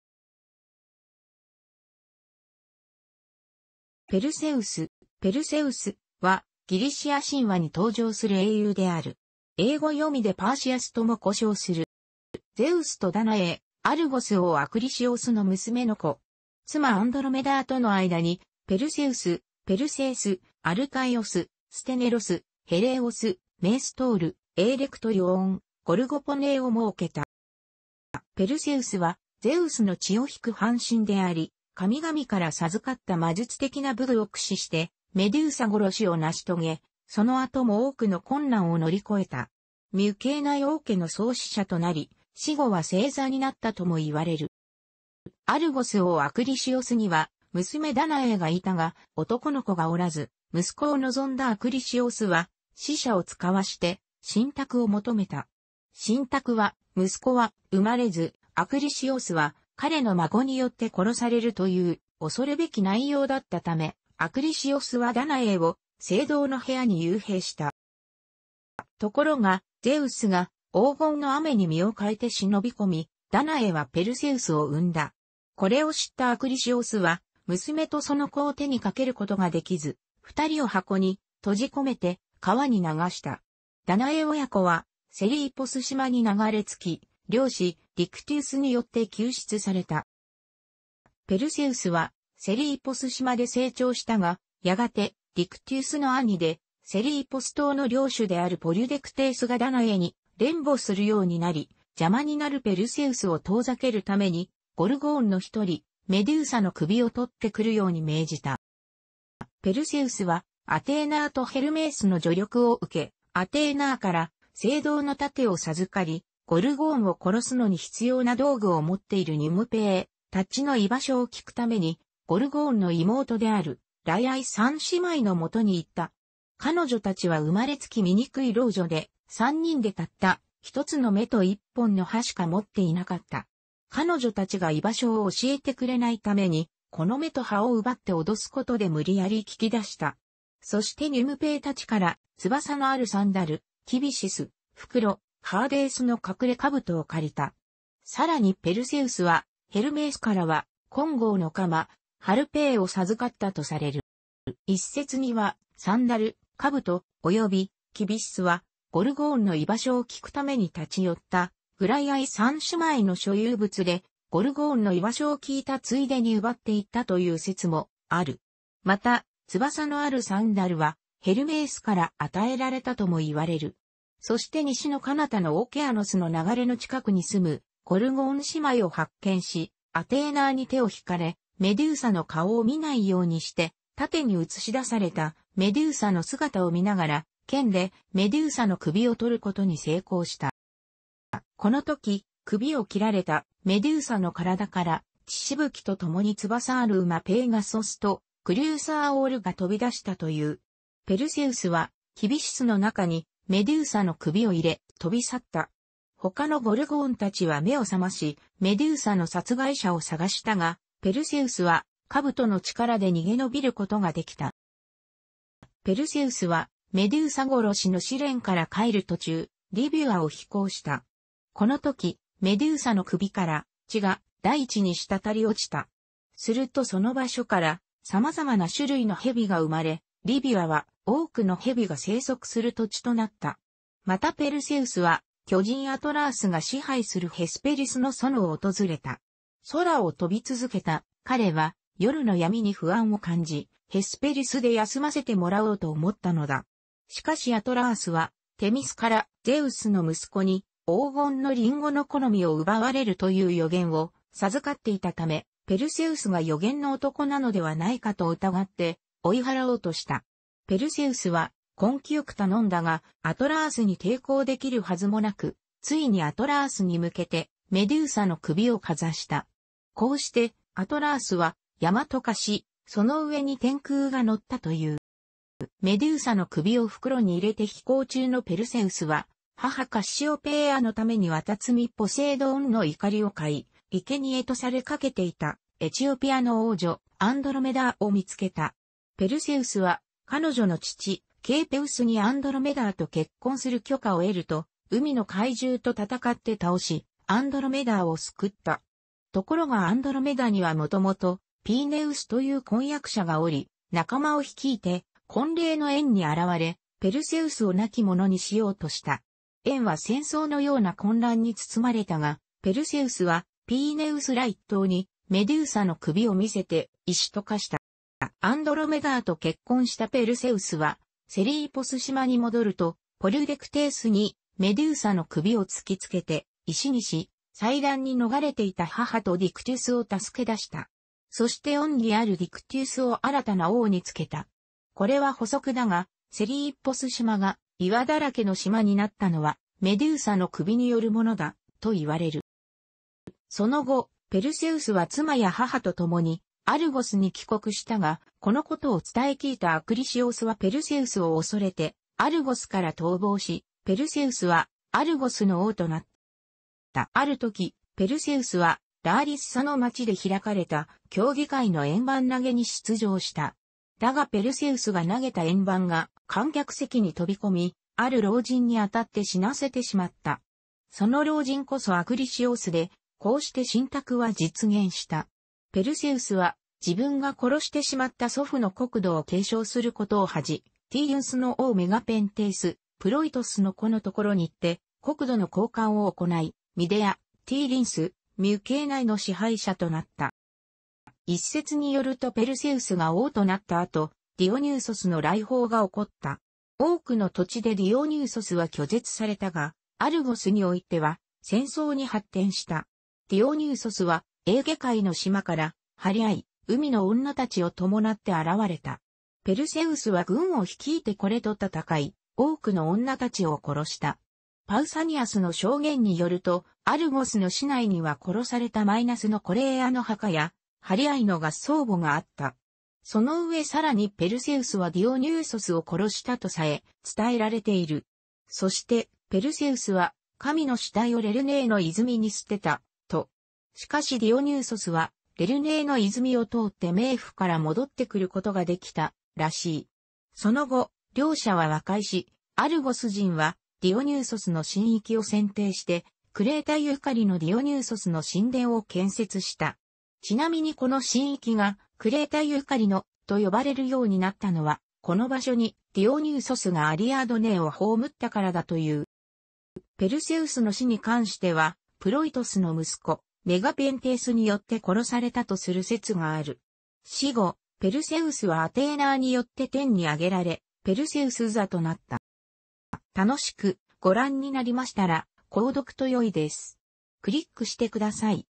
ペルセウス
ペルセウス（, ）は、ギリシア神話に登場する英雄である。英語読みでパーシアスとも呼称する。

ゼウスとダナエー（アルゴス王アクリシオスの娘）の子。妻アンドロメダーとの間にペルセウス（ペルセース）、アルカイオス、ステネロス、ヘレイオス、メーストール、エーレクトリュオーン、ゴルゴポネーをもうけた。

ペルセウスはゼウスの血を引く半神であり、神々から授かった魔術的な武具を駆使してメドゥーサ殺しを成し遂げ、その後も多くの困難を乗り越えた。ミュケーナイ王家の創始者となり、死後は星座になったとも言われる。

アルゴス王アクリシオスには娘ダナエーがいたが、男の子がおらず、息子を望んだアクリシオスは使者を使わして神託を求めた。神託は「息子は生まれず、アクリシオスは彼の孫によって殺される」という恐るべき内容だったため、アクリシオスはダナエーを青銅の部屋に幽閉した。

ところがゼウスが黄金の雨に身を変えて忍び込み、ダナエーはペルセウスを産んだ。これを知ったアクリシオスは、娘とその子を手にかけることができず、二人を箱に閉じこめて川に流した。ダナエー親子はセリーポス島に流れ着き、漁師ディクテュスによって救出された。

ペルセウスはセリーポス島で成長したが、やがて、ディクテュスの兄でセリーポス島の領主であるポリュデクテースがダナエーに恋慕するようになり、邪魔になるペルセウスを遠ざけるためにゴルゴーンの一人メドゥーサの首を取ってくるように命じた。

ペルセウスはアテーナーとヘルメースの助力を受け、アテーナーから青銅の盾を授かり、ゴルゴーンを殺すのに必要な道具を持っているニュムペーたちの居場所を聞くためにゴルゴーンの妹であるグライアイ三姉妹の元に行った。彼女たちは生まれつき醜い老女で、三人でたった一つの眼と一本の歯しか持っていなかった。彼女たちが居場所を教えてくれないために、この眼と歯を奪って脅すことで無理やり聞き出した。そしてニュムペーたちから翼のあるサンダル、キビシス（袋）、ハーデースの隠れ兜を借りた。さらにペルセウスはヘルメースからは金剛の鎌（ハルペー）を授かったとされる。

一説には、サンダル、兜、およびキビシスはゴルゴーンの居場所を聞くために立ち寄ったグライアイ三姉妹の所有物で、ゴルゴーンの居場所を聞いたついでに奪っていったという説もある。また、翼のあるサンダルはヘルメースから与えられたともいわれる。そして西の彼方のオーケアノスの流れの近くに住むゴルゴーン姉妹を発見し、アテーナーに手を引かれ、メドゥーサの顔を見ないようにして、盾に映し出されたメドゥーサの姿を見ながら、剣でメドゥーサの首を取ることに成功した。

このとき、首を切られたメドゥーサの体から血しぶきとともに翼ある馬ペーガソスとクリューサーオールが飛び出したという。ペルセウスはキビシスの中にメドゥーサの首を入れ、飛び去った。他のゴルゴーンたちは目を覚まし、メドゥーサの殺害者を探したが、ペルセウスは兜の力で逃げのびることができた。

ペルセウスはメドゥーサ殺しの試練から帰る途中、リビュアを飛行した。このときメドゥーサの首から血が大地に滴り落ちた。するとその場所から様々な種類のヘビが生まれ、リビュアは多くのヘビが棲息する土地となった。またペルセウスは巨人アトラースが支配するヘスペリスの園を訪れた。空を飛び続けた彼は夜の闇に不安を感じ、ヘスペリスで休ませてもらおうと思ったのだ。しかしアトラースはテミスから「ゼウスの息子に黄金の林檎の木の実を奪われる」という予言を授かっていたため、ペルセウスが予言の男なのではないかと疑って追い払おうとした。ペルセウスは根気よく頼んだが、アトラースに抵抗できるはずもなく、ついにアトラースに向けてメドゥーサの首をかざした。こうしてアトラースは山と化し、そのうえに天空が乗ったという。

メドゥーサの首を袋に入れて飛行中のペルセウスは、母カッシオペイアのために海神ポセイドーンの怒りを買い、生贄とされかけていたエチオピアの王女アンドロメダーを見つけた。ペルセウスは彼女の父ケーペウスにアンドロメダーと結婚する許可を得ると、海の怪獣と戦って倒し、アンドロメダーを救った。ところがアンドロメダにはもともとピーネウスという婚約者がおり、仲間を率いて婚礼の宴に現れ、ペルセウスを亡き者にしようとした。宴は戦争のような混乱に包まれたが、ペルセウスはピーネウスら一党にメドゥーサの首を見せて石と化した。

アンドロメダーと結婚したペルセウスはセリーポス島に戻ると、ポリュデクテースにメドゥーサの首を...